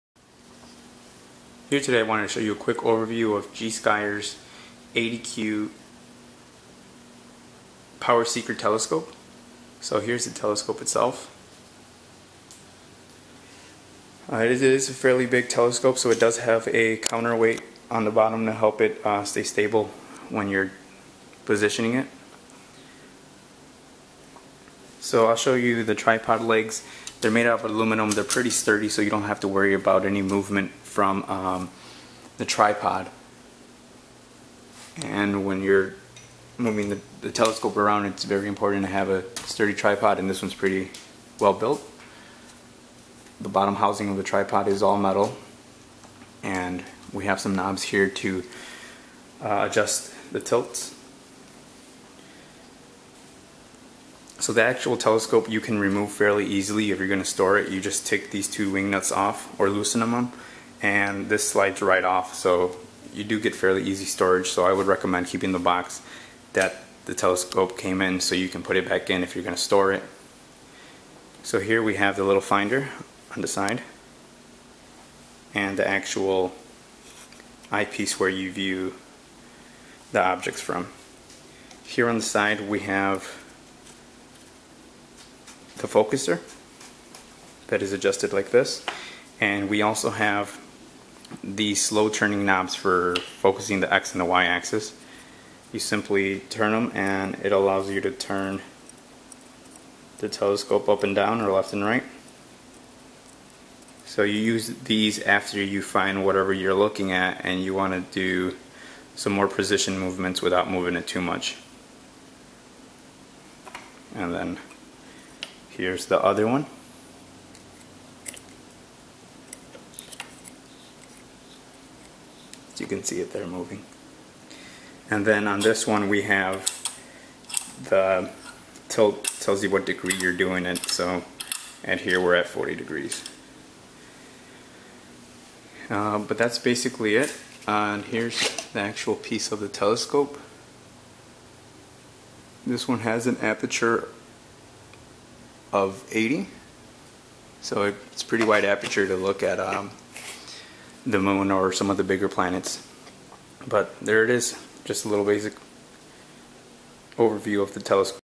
Gskyer Astronomy Instruments 80EQ PowerSeeker Telescope Overview
Gskyer® Astronomy Instruments 80EQ PowerSeeker Telescope

Product description:

100%New, Specialized supply for scientists to research professional astronomy and Natural Science
German equatorial mount,and Comes with Stainless Steel Tripod and accessory tray
Aperture: 80mm(3.2"). Focal Length: 900mm. Focal Ratio: f/11.25. Rack-and-Pinion Focuser &Low (25mm) , medium(10mm), high (5mm) magnification eyepieces give you variety for any viewing situation & 3x barlow lens doubles the magnifying power of each eyepiece
All-glass optical components with high transmission coatings for enhanced image brightness and clarity and stars, with detailed instructions for our every customer, it is very easy and quick assemble. And become favorite by every one
Lifetime warranty ,we will provide the best service and reliable products to our every customer. And best quality at this reasonable price,please ease to use